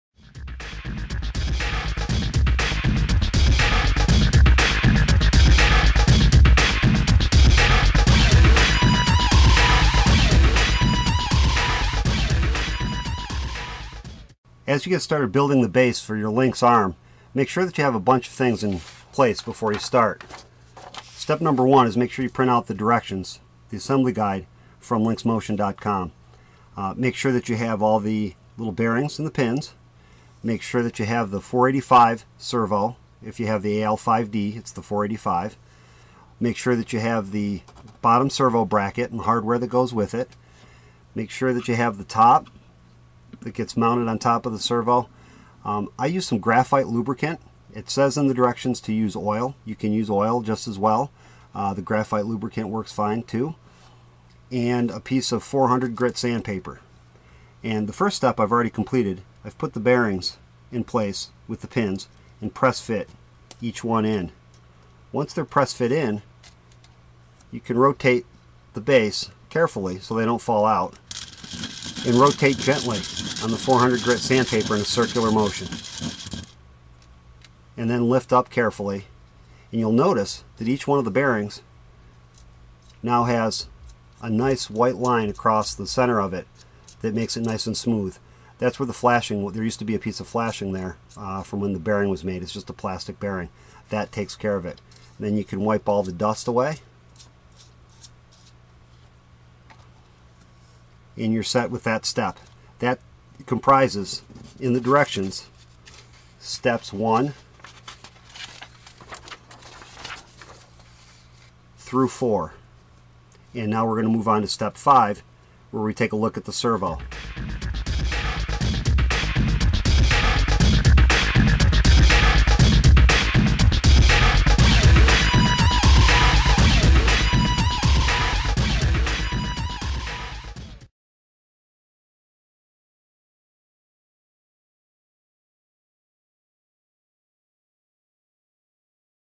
Lynx Arm: Mechanical Assembly
Step by step directions following the assembly

Tips and tricks

too!

Get the whole 32 minute video at

Building the Base:
00:14.33 Pin/Bearings/Sanding
01:59.01 Base servo information
03:44.62 Installing the base servo
04:30.60 Attach Servo Bracket
06:24.53 Using thread lock compound
08:01.92 Lubrication/Alignment/Final Assembly

Building the Arm
10:34.33 Lexan Prep & C Brackets
11:41.62 Friction Plates
14:03.16 Install Arm Pivot
14:42.77 Attach C Brackets to Base
15:43.27 Install Shoulder Servo
17:17.66 Hubs/Tubing/Angle brackets
17:40.98 Attaching Wrist & Elbow Multipurpose

brackets
19:02.81 Install Elbow and wrist servo
22:43.42 Hard to reach nuts
24:19:28 Servo Horn Alignment
26:51.45 Adjusting Friction Plates
28:02.49 Attaching Load Balance Springs
29:23.00 Building the Lightweight Wrist Rotate